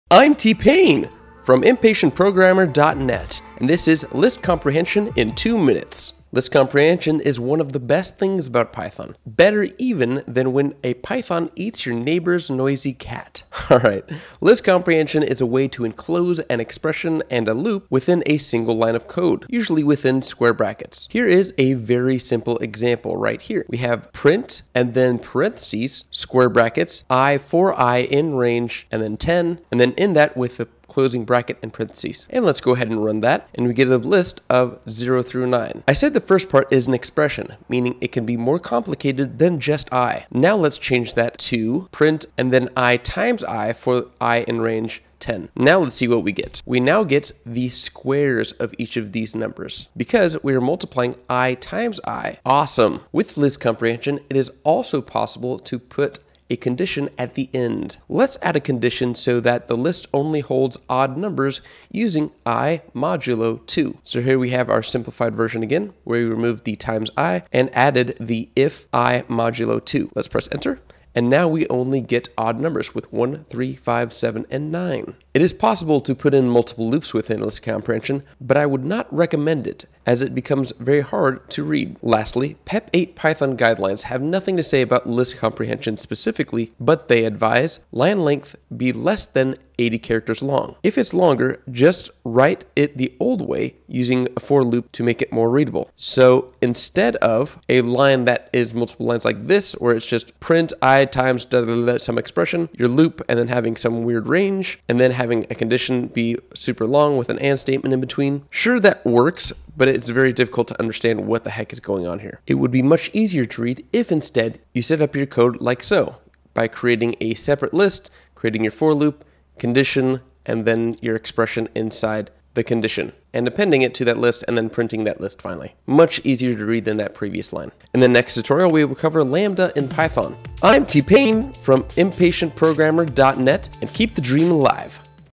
Python | List Comprehension in 2 minutes
This two minute Python scripting tutorial is focused on List comprehension. We can condense an expression, a loop and condition into a single line of code easily using List Comprehension, one of the coolest features of python.

INTERMEDIATE EPISODES:

♦ Impatient Programmer

New Episode (nearly) Every Friday

 - Marcus G.
 - JK
 - Yu-Chiang H.

My comments and words are my own and do not represent the views or opinions of my employer in anyway.